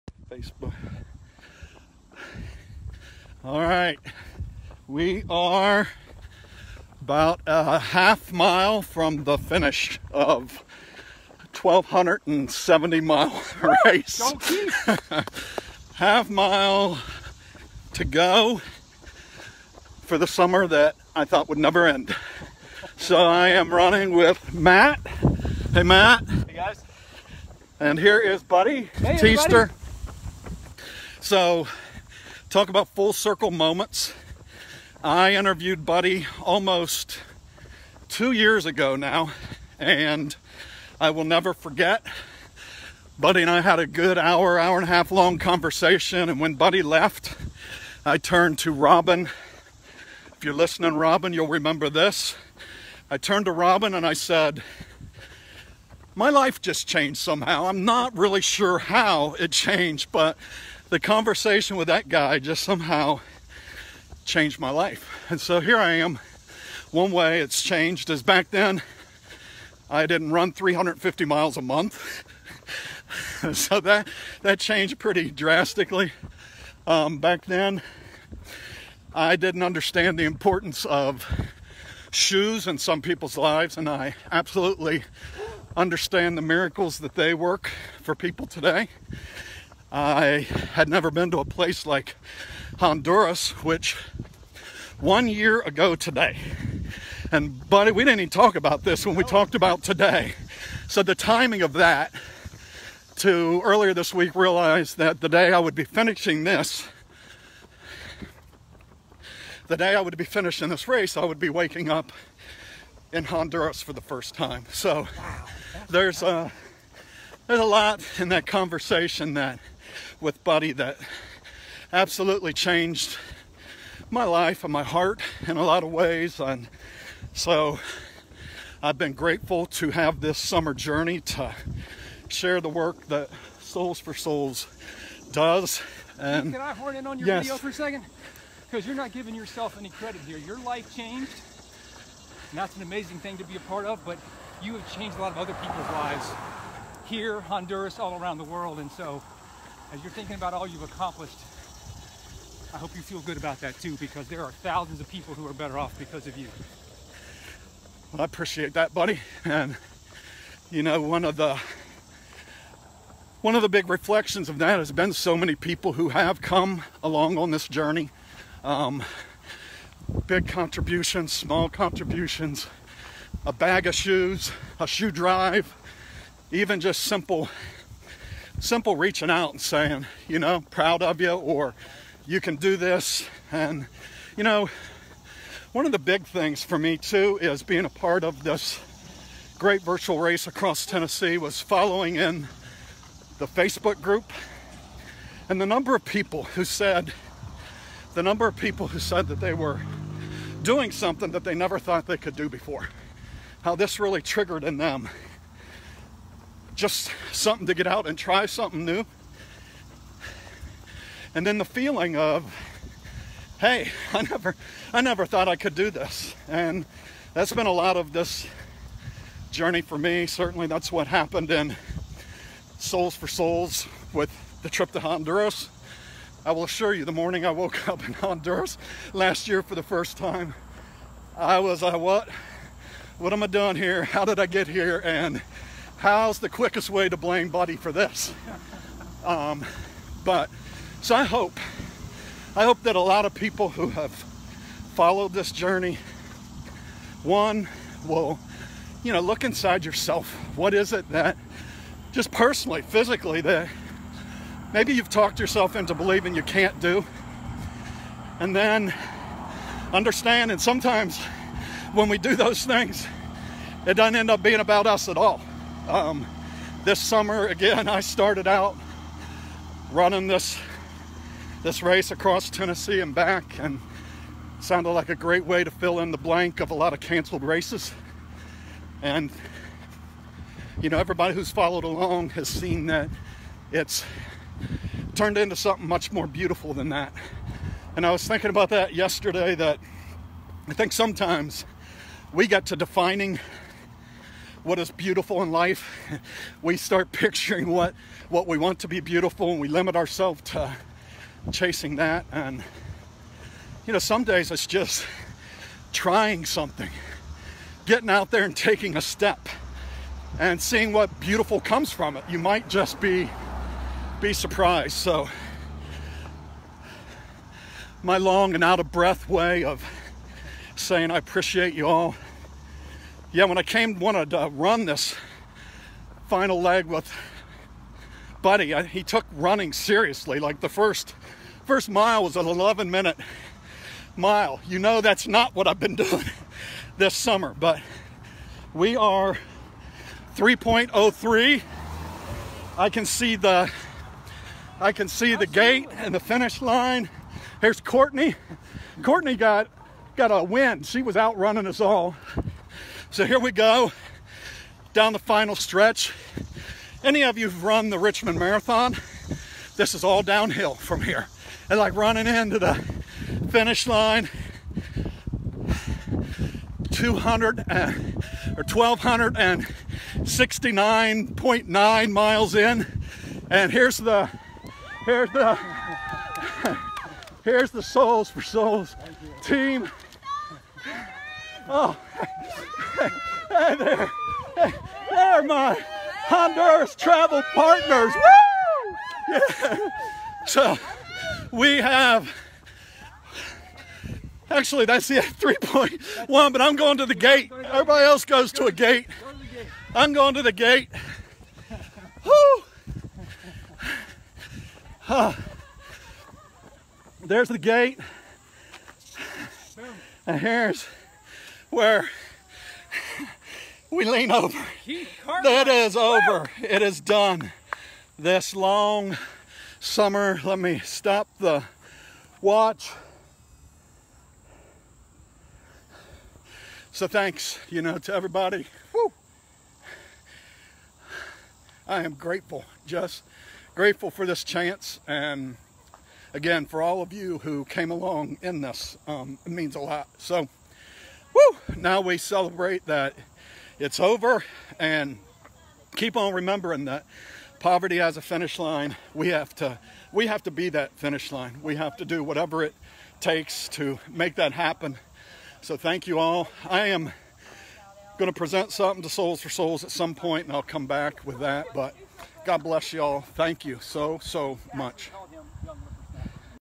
Running the final half mile of the Great Virtual Race Across Tennessee in Nashville, Tennessee.
During the summer of 2020, I ran the 1,270 mile Great Virtual Race Across Tennessee and back again. The race was to raise money for Soles4Souls. We ultimately raised over $6,000. I went to Nashville, Tennessee on August 28th and ran the final 1/2 mile of the race with Soles4Souls CEO Buddy Teaster.